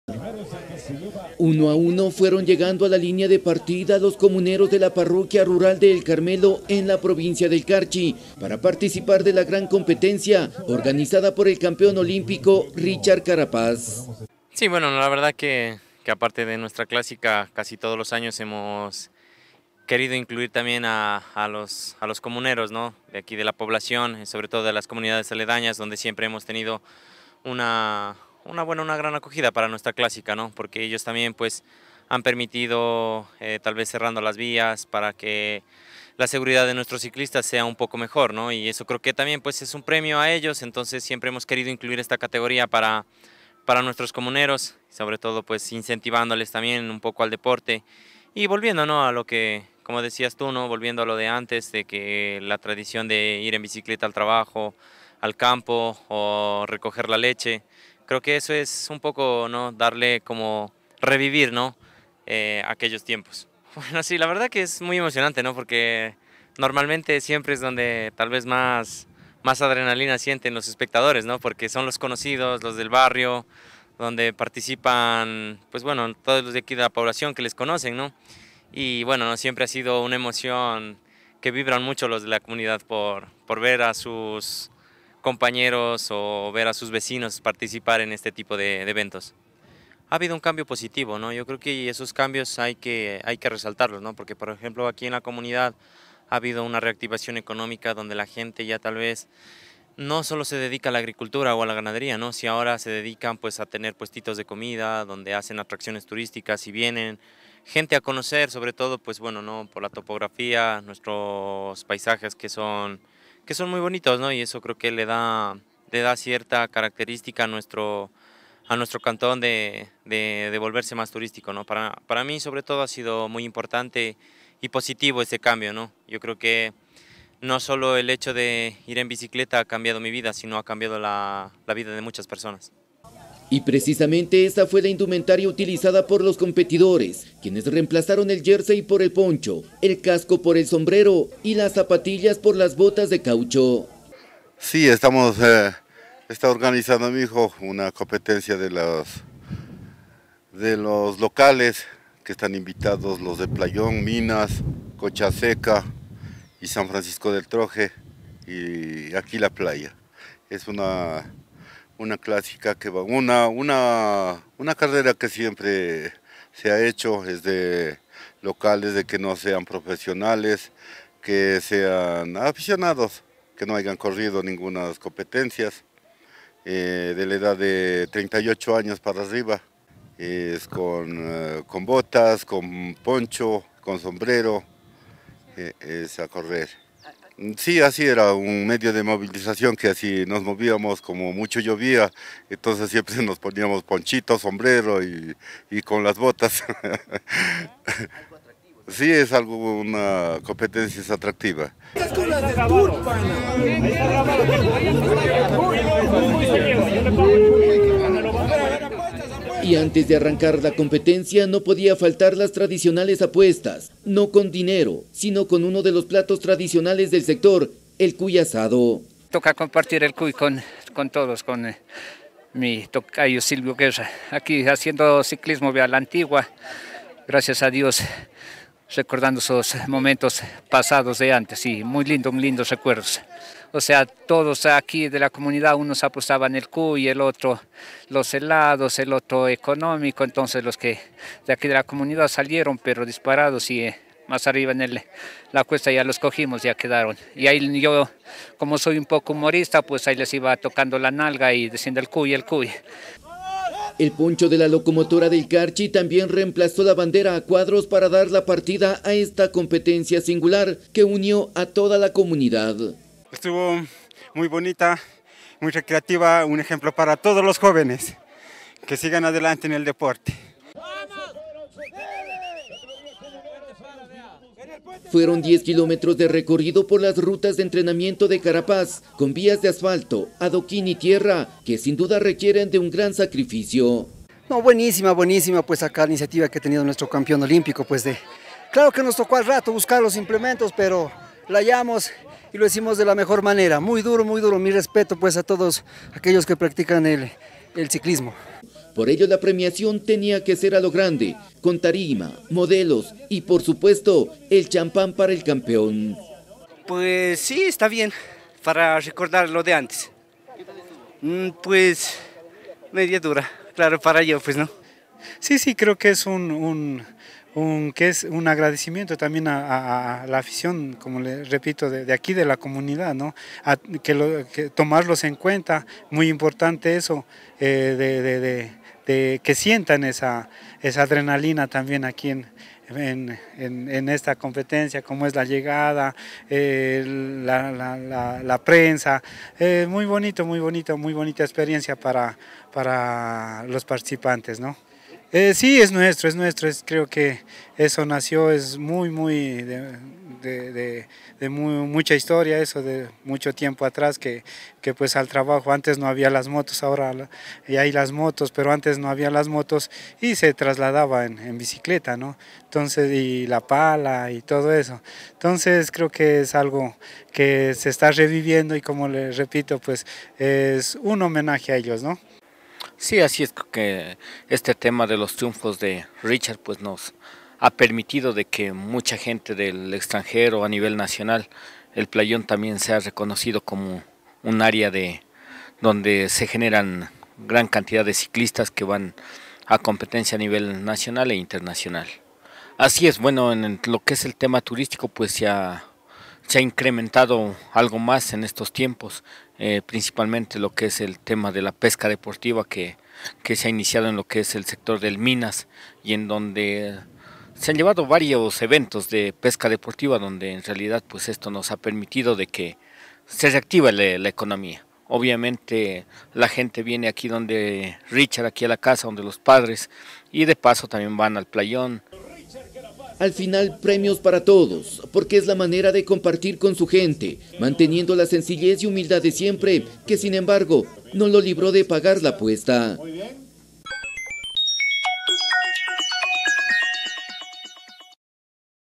RICHARD CARAPAZ ORGANIZÓ LA COMPETENCIA CICLÍSTICA DE LOS COMUNEROS
El campeón olímpico Richard Carapaz, antes de su viaje a España para su primera concentración con el nuevo equipo, organizó una carrera ciclística singular en su comunidad.

Los vecinos de Playa Alta en la parroquia El Carmelo, compitieron con botas, poncho y sombrero.